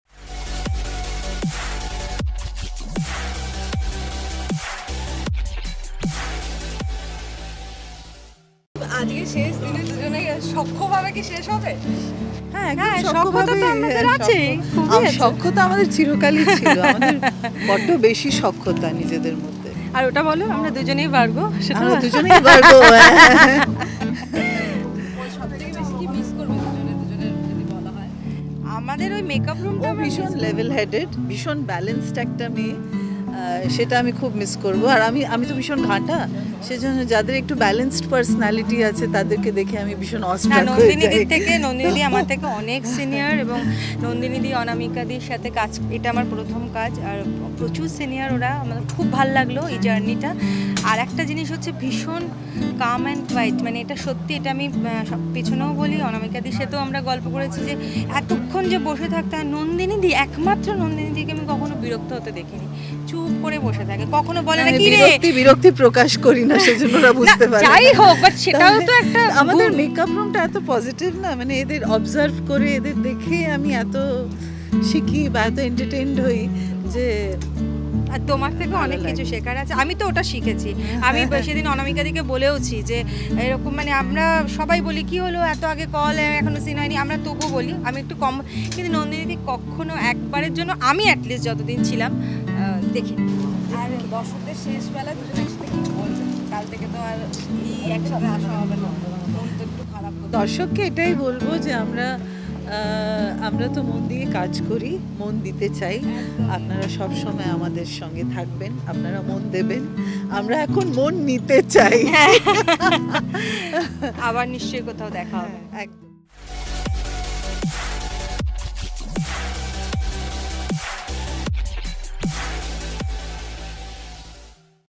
শেষ দিন মালিনী কি বলল মিতাকে ? Malini | Mita | Mon Dite Chai | Last Day | Zee Bangla
মালিনী আর মিতার দ্বন্দ্ব দেখতে কিন্তু দর্শকরা খুবই পছন্দ করেন. তারা তাদের অভিনয়ের মধ্য দিয়ে দর্শকদের মন জয় করে নিয়েছেন এই অল্প সময়ের মধ্যেই। তাদের বন্ডিং চোখে পড়ার মতন অফ ক্যামেরা। দুজনে দুজনের প্রশংসা করলেন শেষ দিনএ। দুজনে দুজননের সাথে কাটানো ভালো সময় গুলো তুলে ধরলেন আমাদের সামনে। আর তারা একে অপরকে যে মিস করবে সেই সম্পর্কেও জানালেন আমাদেরকে।